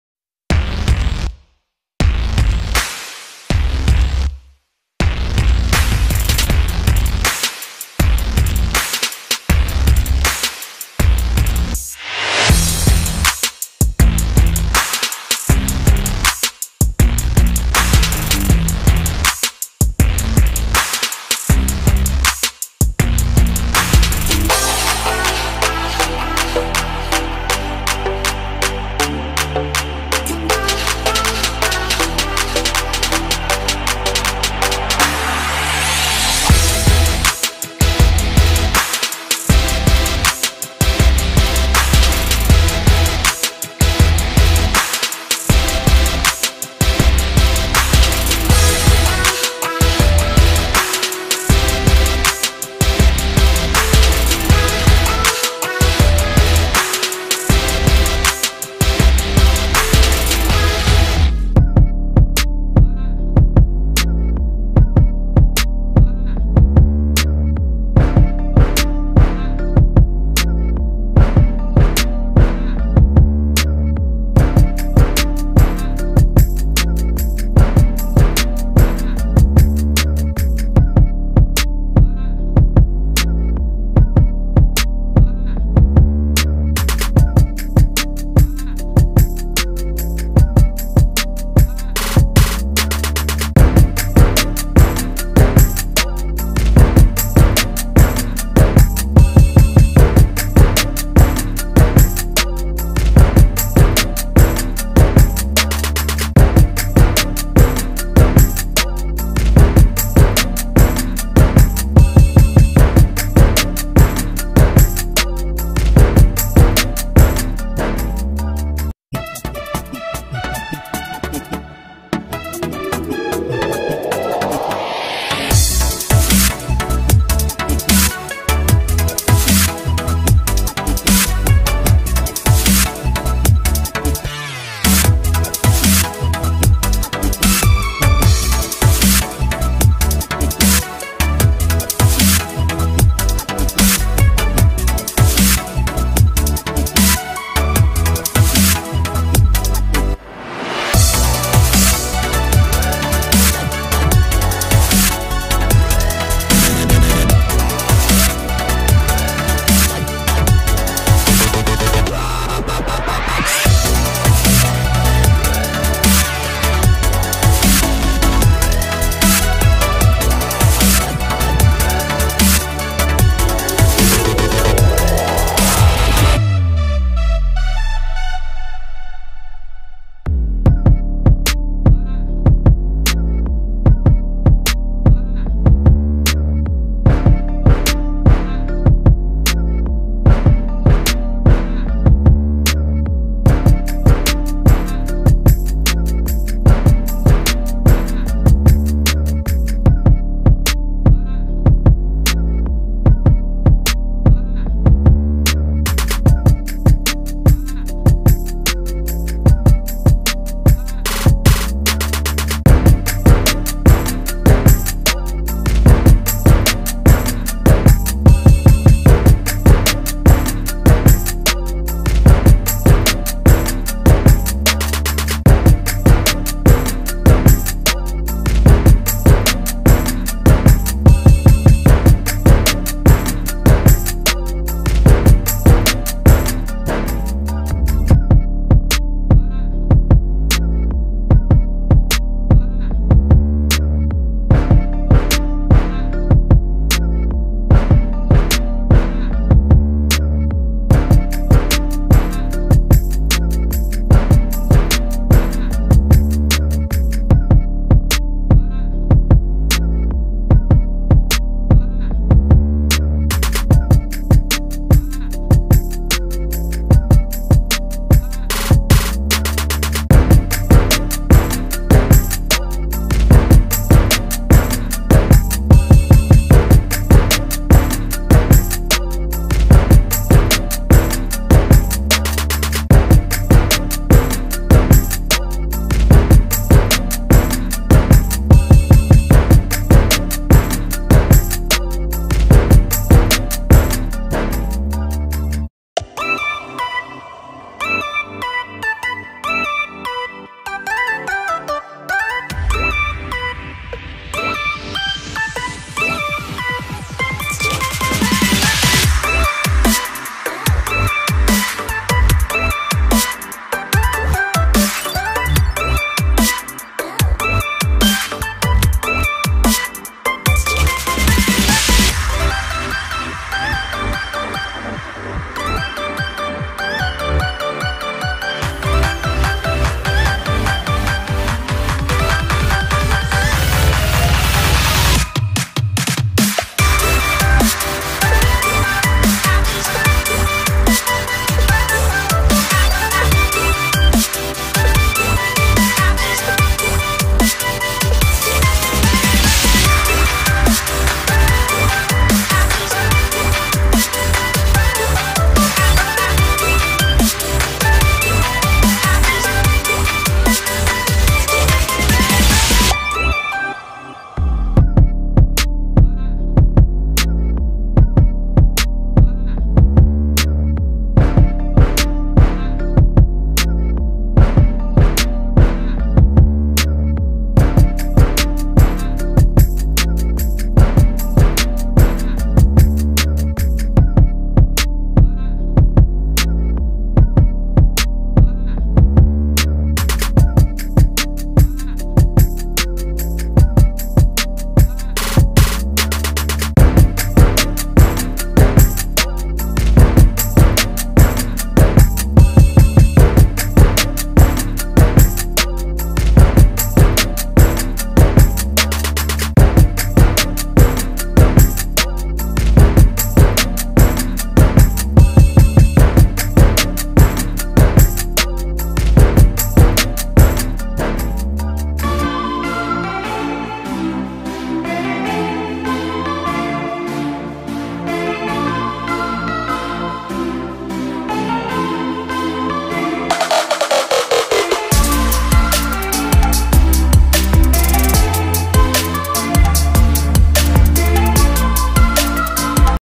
FMS Futura 80mm Jet Flight:Morning, Flushing meadow Corona park Model Airplane Field, Aerobatics.
6s 5000mah